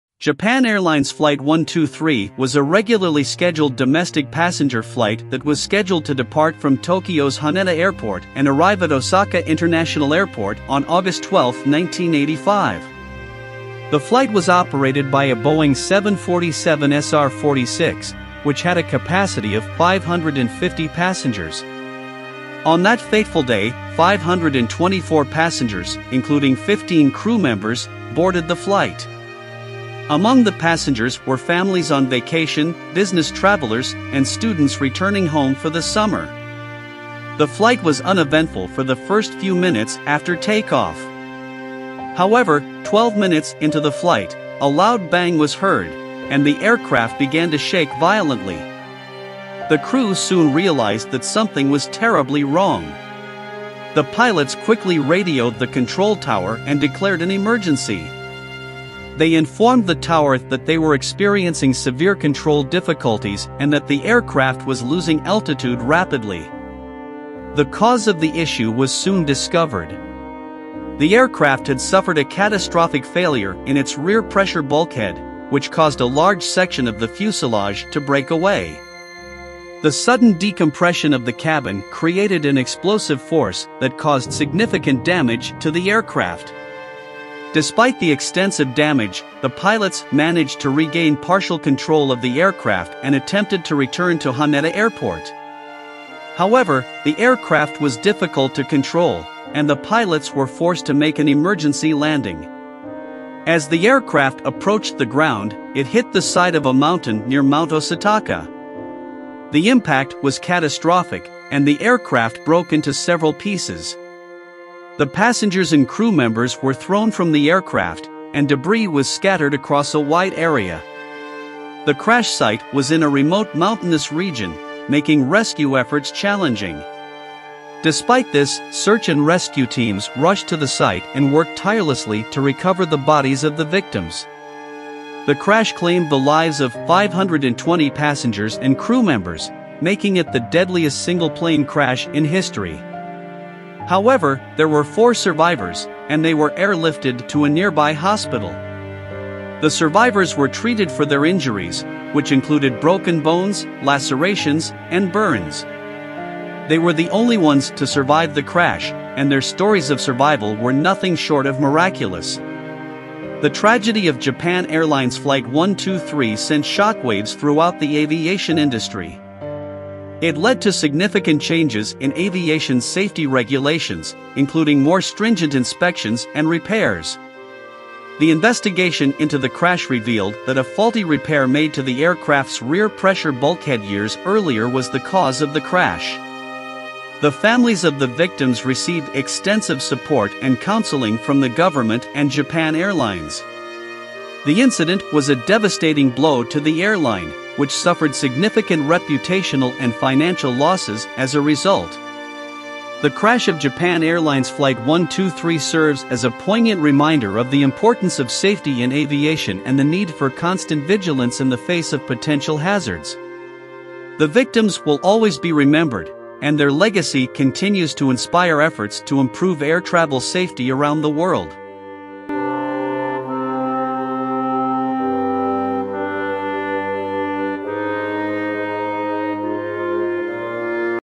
520 Dead, 4 Miraculous Survivors: The Story of Japan Airlines Flight 123
520 Dead, 4 Miraculous Survivors: The Story of Japan Airlines Flight 123 | Airplane Pilot Life

What would you do if you were the pilot of a Japan Airlines airplane when it exploded in mid-air? This is the story of 520 dead and 4 miraculous survivors, as told by the pilot himself.

Japan Airlines Flight 123 was en route from Tokyo to Osaka when it exploded in mid-air, resulting in the tragic deaths of 520 passengers and crew. In this video, the pilot of the aircraft tells the story of the event from start to finish. This video is a must-watch for anyone interested in aviation, or Japanese history.